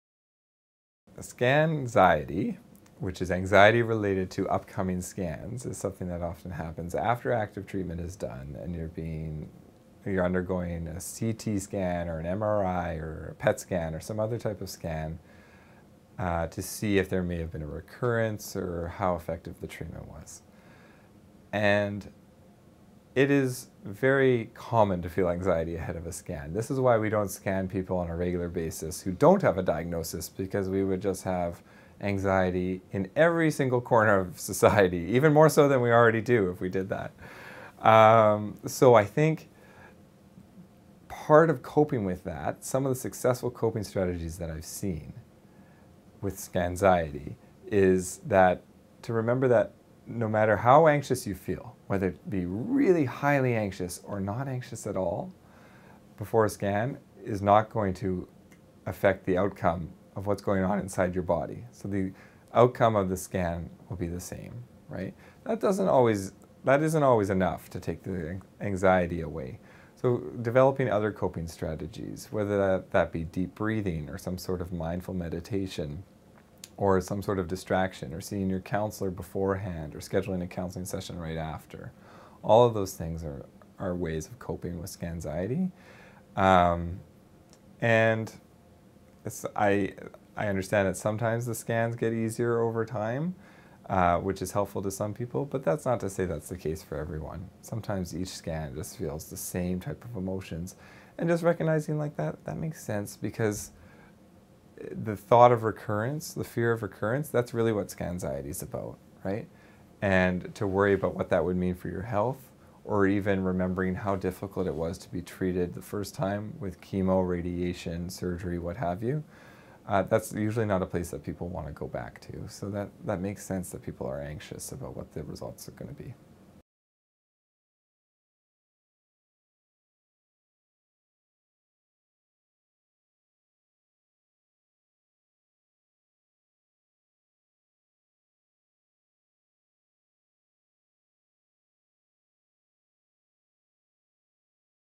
Coping with "scanxiety"
In this video by Canadian Virtual Hospice, Ian Scott, MSW, RSW talks about the anxiety that comes with having scans and tests and provides some helpful coping strategies.